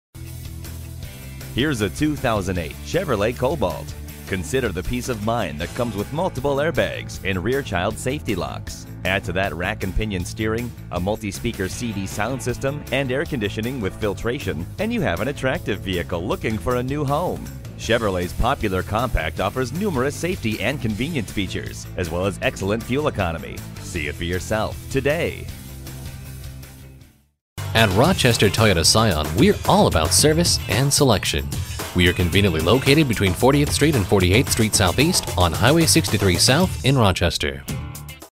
2008 Chevrolet Cobalt AT17112 in Rochester Minneapolis, MN - SOLD
SOLD - If you are looking for real value on a great used car, Rochester Toyota invites you to call and make an appointment to view this 2008 Chevrolet Cobalt, stock# AT17112. We are conveniently located near Rochester Minneapolis, MN and known for our great selection, reliability and quality. Call to take a look at this 2008 Chevrolet Cobalt today.

Rochester Toyota
4900 Highway 52 N
Rochester Minneapolis MN, 55901